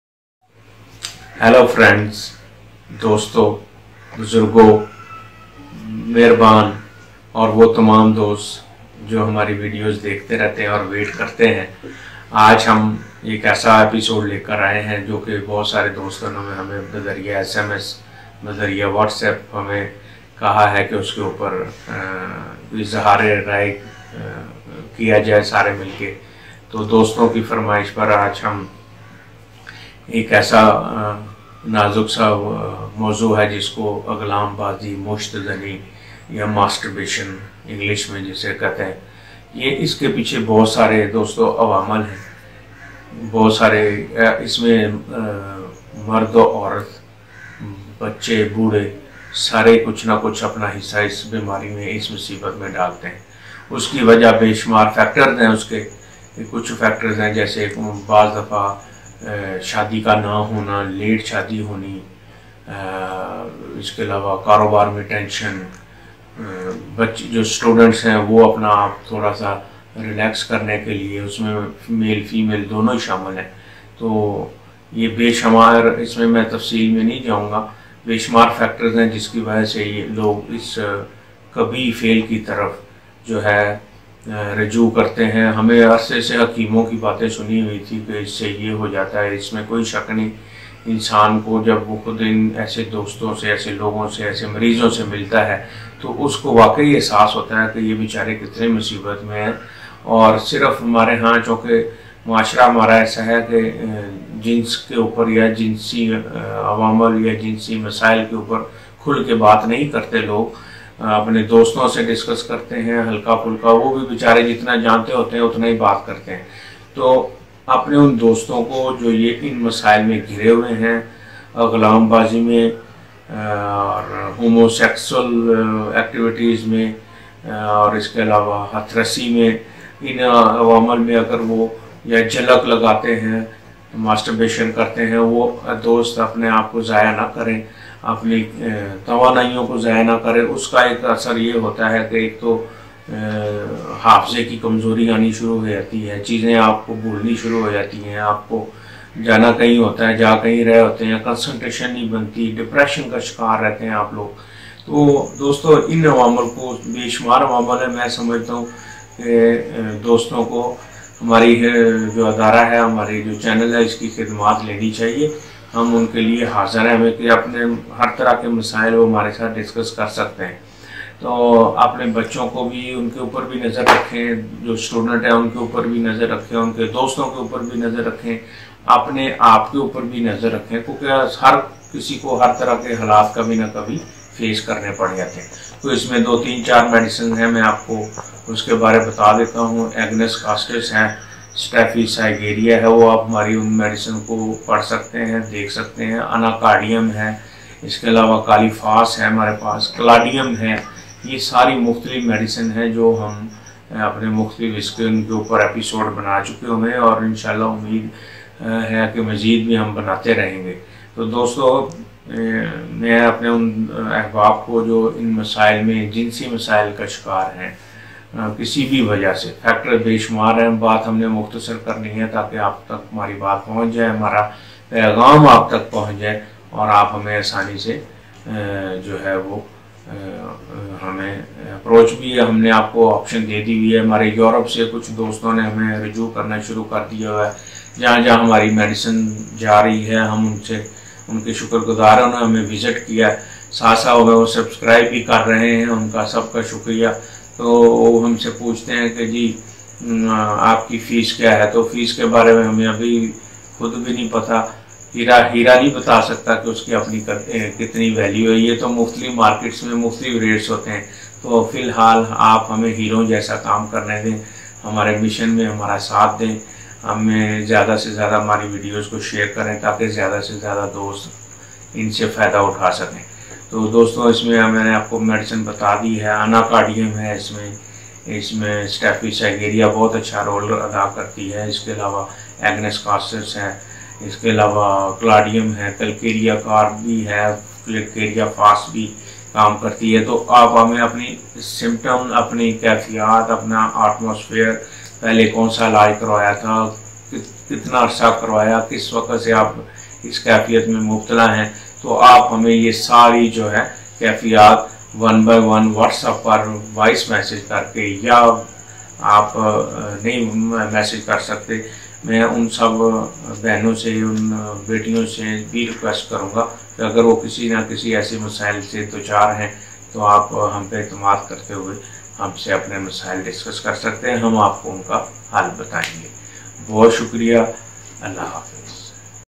Musht Zani ka Ilaj In Urdu
Dosto Masturbation Musht Zani se bachne ki Medicine note karlen

HOMEOPATHIC MEDICINE NAME IS

AGNUS CASTUS

CALC CARB

CALC PHOS

dosto ye medicine ap mujhe whatsapp pr symptoms discuss kar ky use karni hen...Shukriya